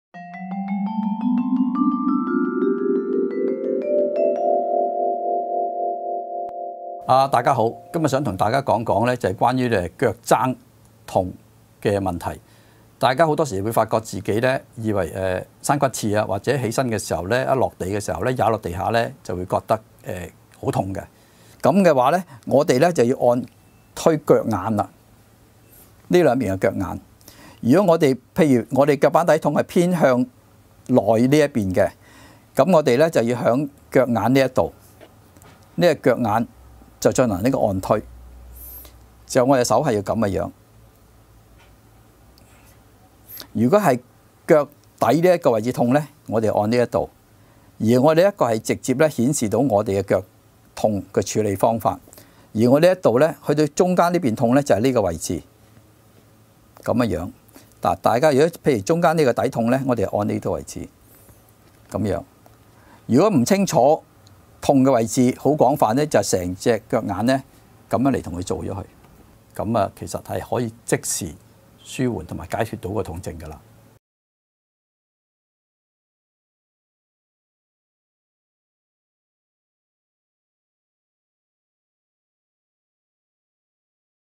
腳踭痛
足底筋膜炎是指足底筋膜這個組織因為足部負擔過重、足底不正確受力及伸張，導致在長期勞損之下而造成的炎症。不論是年輕人、中年人或老年人，均有機會患上足底筋膜炎。教您一招舒缓秘技。